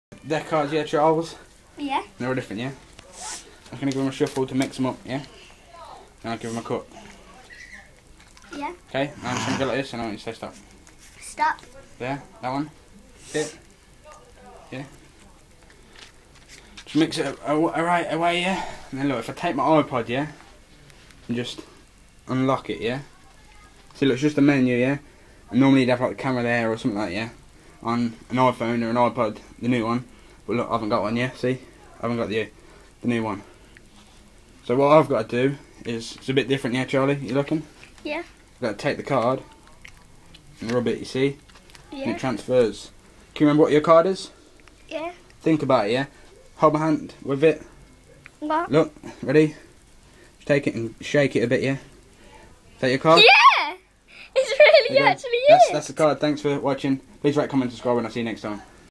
Magic by dan the amazing iPod trick
The iPod transfers the card to the screen